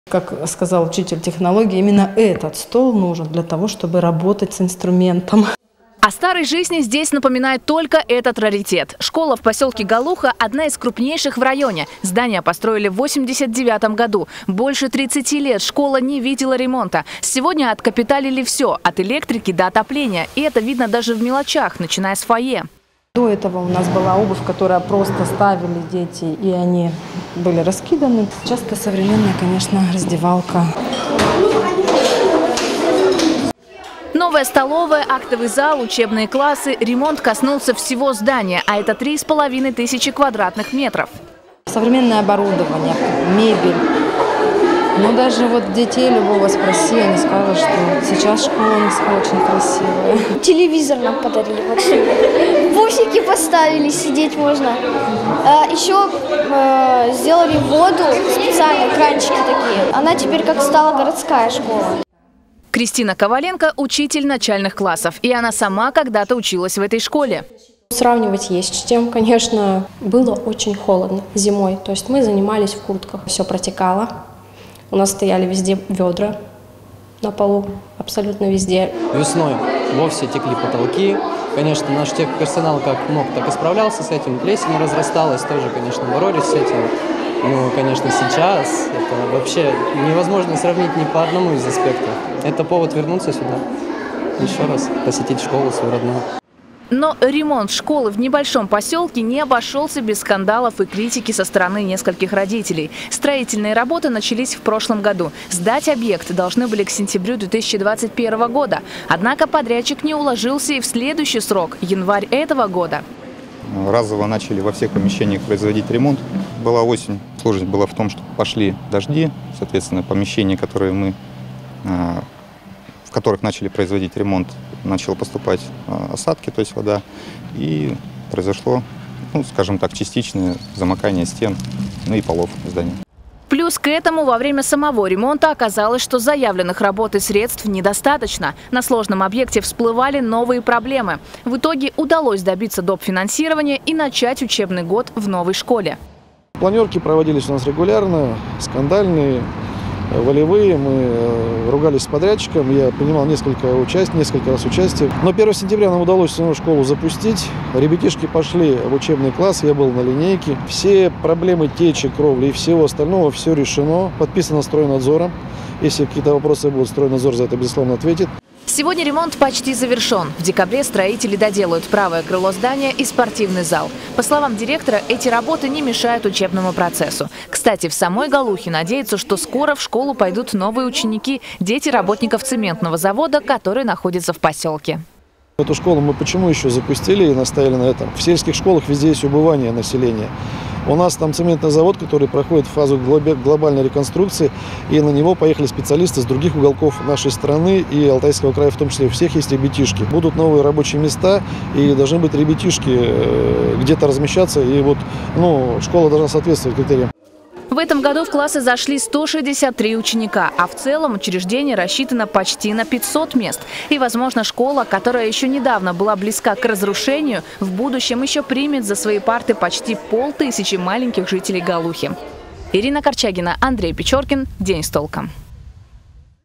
В посёлке Голуха Заринского района начался учебный год в отремонтированной школе
Без плесени и протекающей крыши. В одной из крупнейших школ Заринского района провели долгожданный ремонт. Сегодня в посёлке Голуха обучаются 163 ученика. Но в перспективе школа может принять почти 500 человек